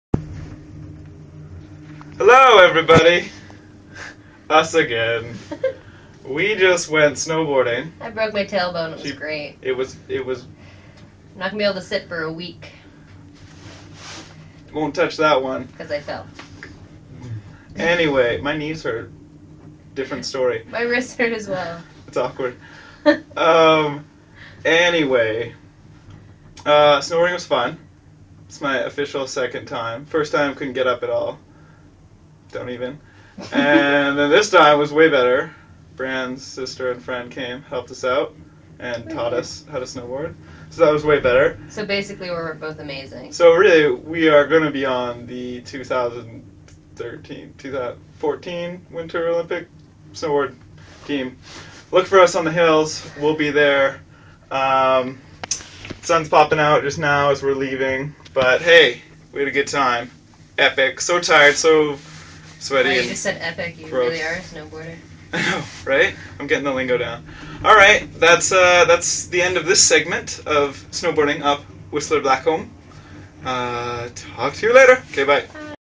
Coming down the Godola after boarding!
I won a contest through First Weekend Club and won a trip for 2 up whistler. These are our stories!